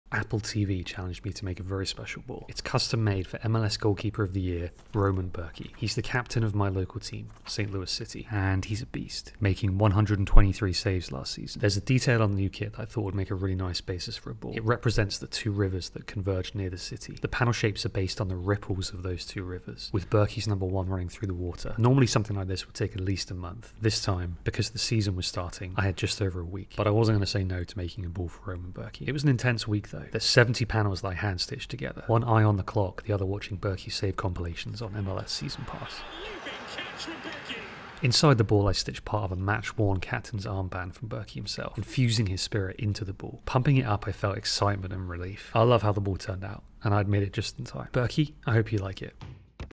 A ball for MLS goalkeeper of the year (Roman Bürki)
I got challenged by @AppleTV to make a unique ball inspired by the captain of my local team, Roman Bürki. Catch him in action all season with MLSSeasonPass.

Want to have a go at making your own ball?